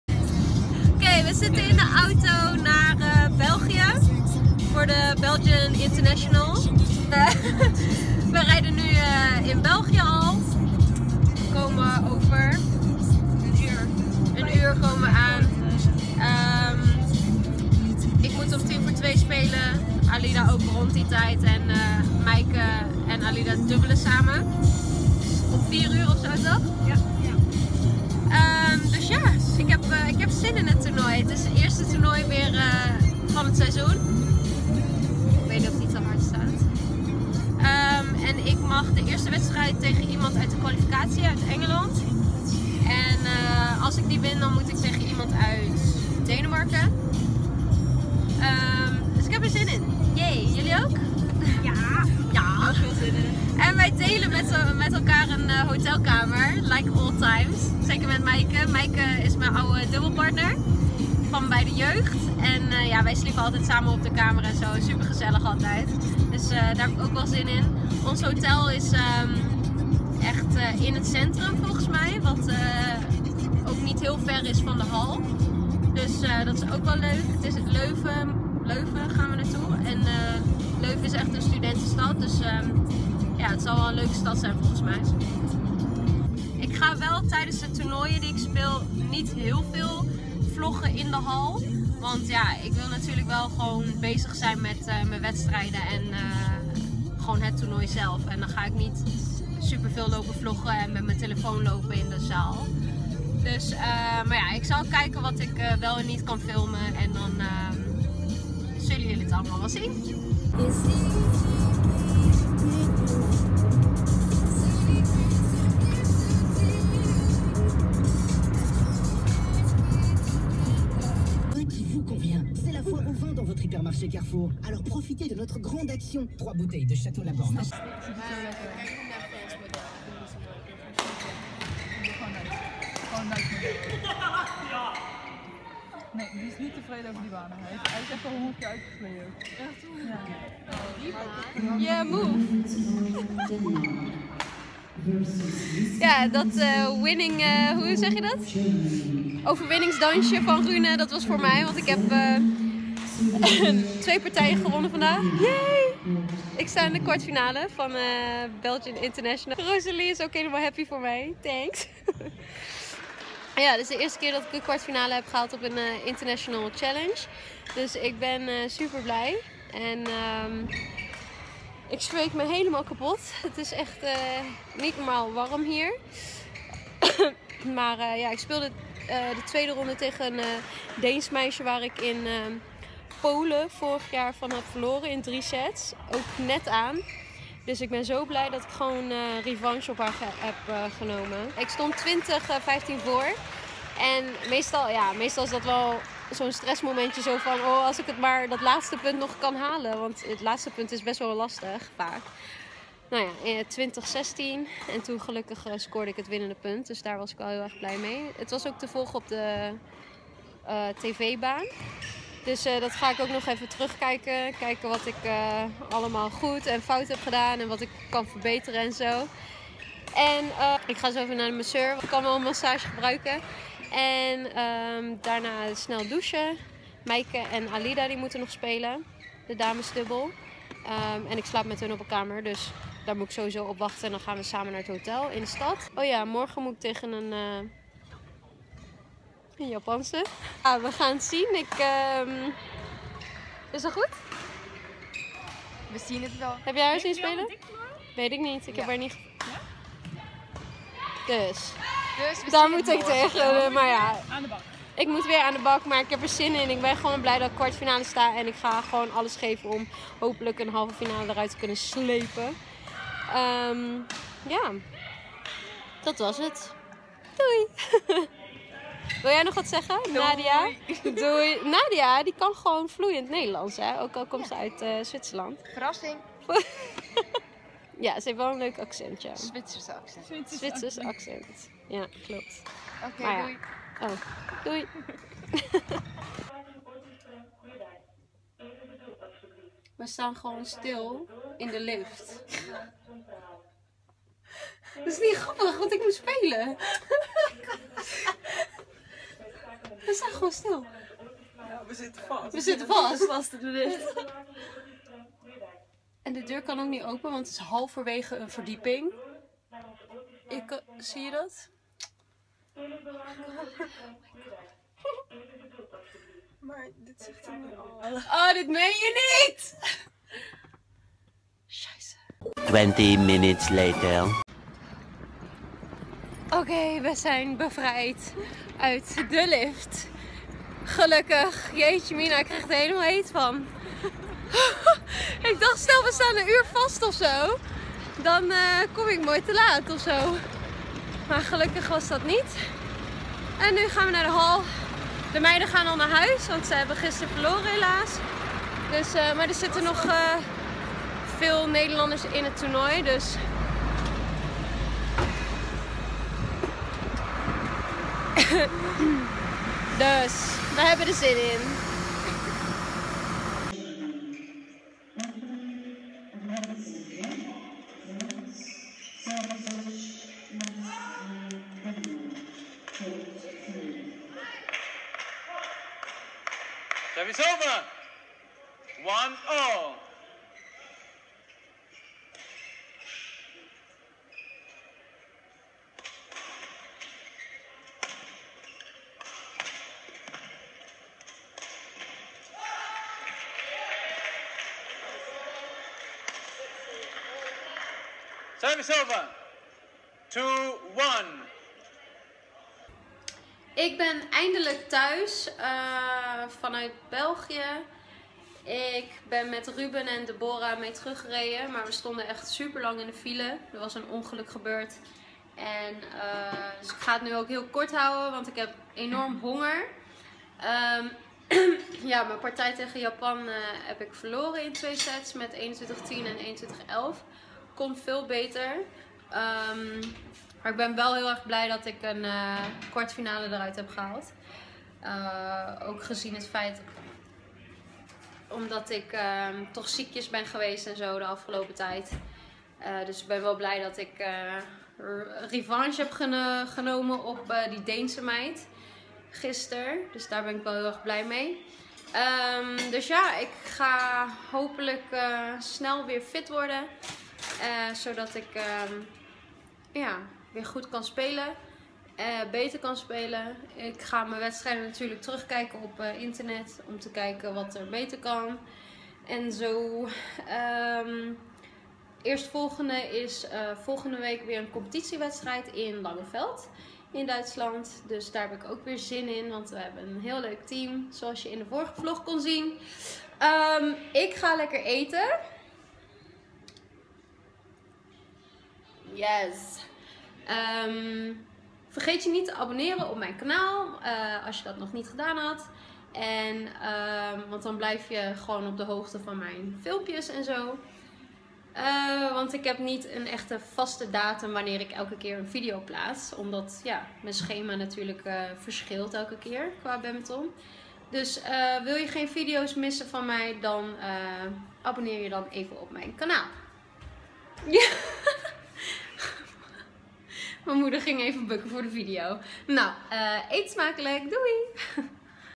Vlog #14 Belgian International
Het eerste toernooi van het seizoen is weer begonnen. Ik reis dit keer naar Leuven, België. En maak weer het één en ander mee, zoals op matchday vast zitten in de hotel lift...